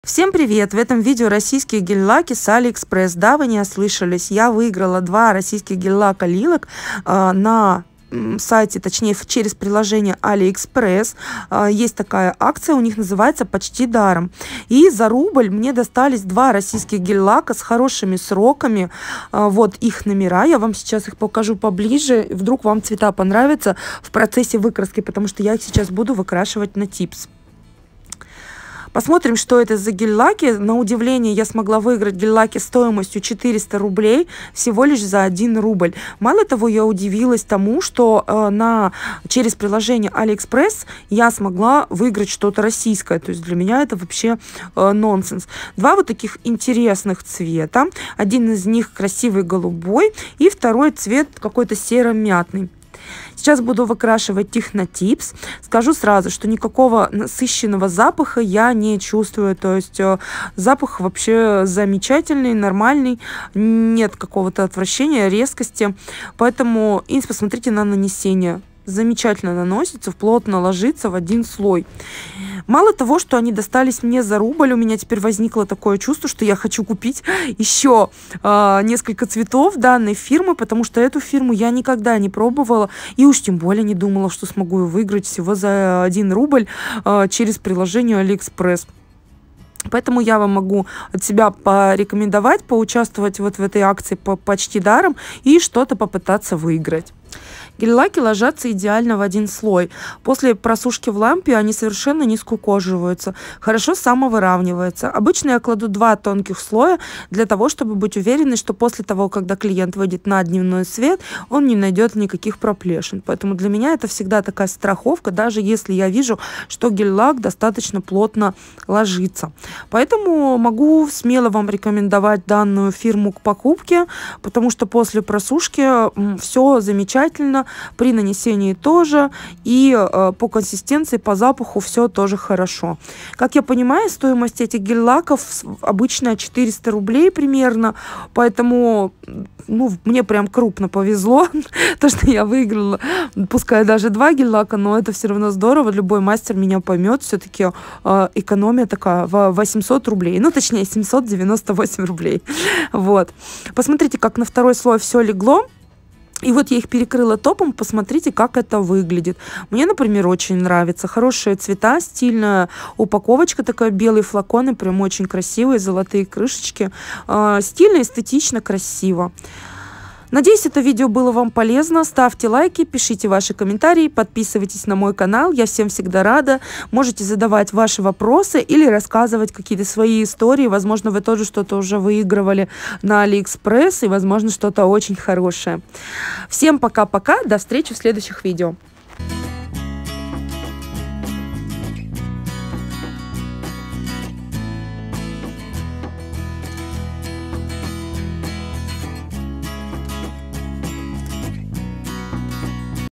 Гель-лаки Lilac с Aliexpress / Выкраска / Почти даром
Всем привет! Меня зовут Александра. Рада приветствовать Вас на своем канале.
Сегодня в видео два гель-лака которые я выиграла через "Почти даром". Эти лаки мне достались за рубль каждый.
Буду рада вашим комментариям, интересны ли вам такие видео?